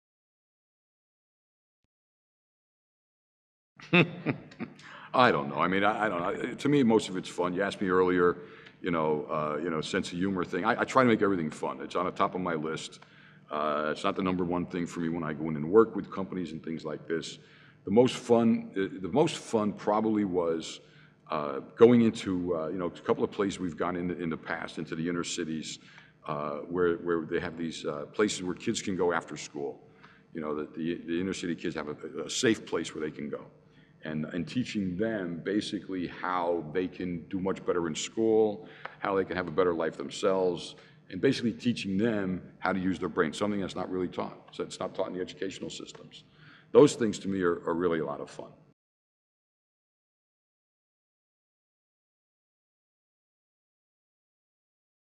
John la Valle. What's the most fun job you have done with NLP?
Interview with John la Valle president from the NLP society. John is one of the best NLP trainers in the world he runs seminars with Richard Bandler throughout the world.